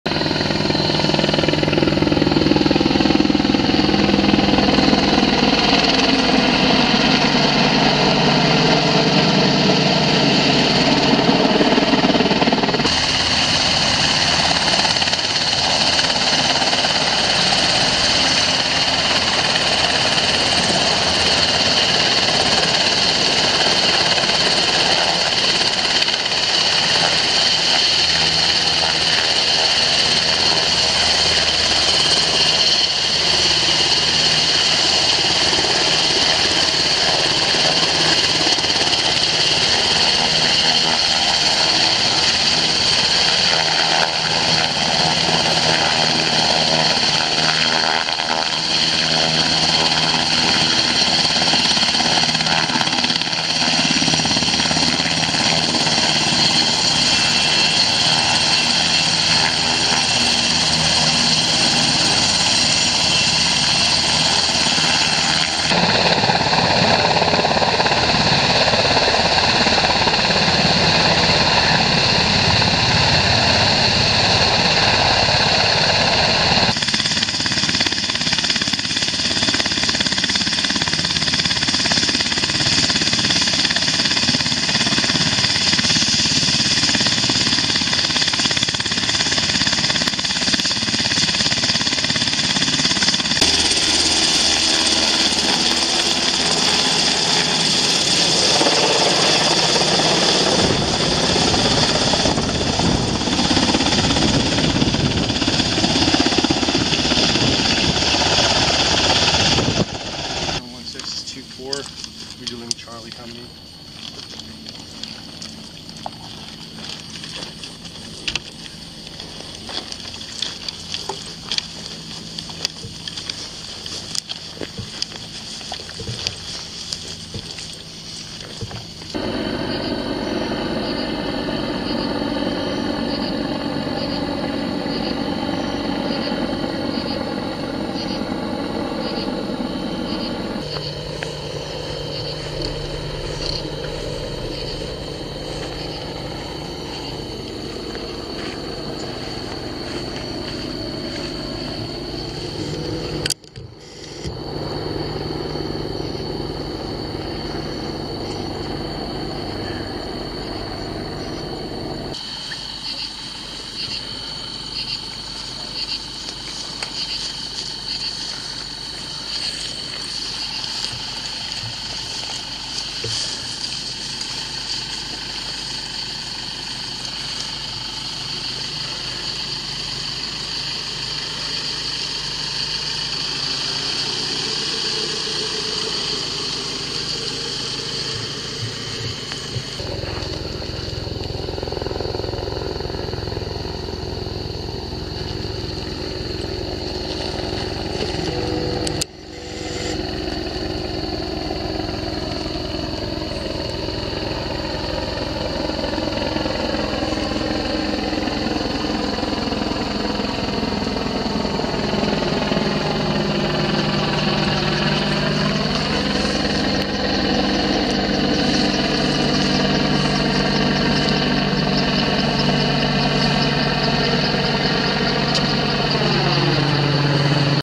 NIGHT AIR ASSAULT with Soldiers of the 101st Airborne Division
Soldiers from the 2nd Mobile Brigade Combat Team, 101st Airborne Division (Air Assault) air assault into the Joint Regional Training Center (JRTC) at Ft. Johnson, LA as part of a large-scale, long range air assault (L2A2) that the 101st launched from Ft. Campbell, KY to JRTC on the night of August 14, 2024. L2A2 means delivering one brigade combat team over 500 nautical miles in one period of darkness, arriving as a cohesive element where the enemy least expects it, capable of fighting behind enemy lines for 14 or more days.